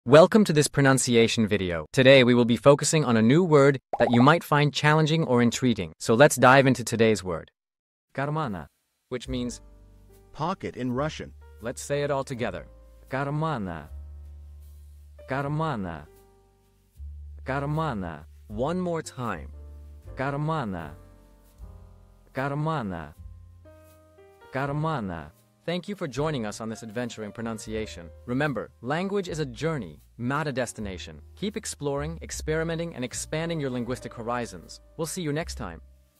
How to pronounce Karmana | Кармана (Pocket in Russian)
Master the Pronunciation of 'Karmana | Кармана' - which means : Pocket in Russian 📝 with @PronunciationRussian 🎓🗣️ - [Your Guide to Russian]

Learn to pronounce 'Karmana' perfectly in Russian with our easy and engaging tutorial by @PronunciationRussian! 📖📽️ In this comprehensive video, we provide you with native speaker examples 🗣️👂 and phonetic breakdowns 🎵 to ensure you get the pronunciation just right.